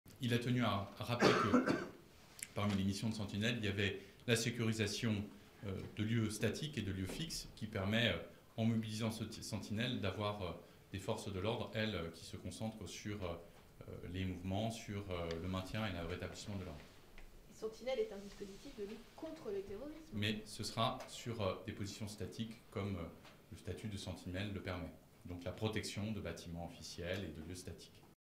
"Gilets jaunes" : les militaires de Sentinelle mobilisés samedi, annonce Griveaux | AFP Extrait
Le porte-parole du gouvernement, Benjamin Griveaux, a expliqué à l'issue du conseil des ministres que la mission antiterroriste militaire Sentinelle serai mobilisée samedi 23 mars de manière renforcée dans le cadre des manifestations des "gilets jaunes" pour protéger des bâtiments officiels et autres "lieux fixes". Objectif du gouvernement : libérer policiers et gendarmes afin qu'ils "se concentrent sur les mouvements, sur le maintien et le rétablissement de l'ordre".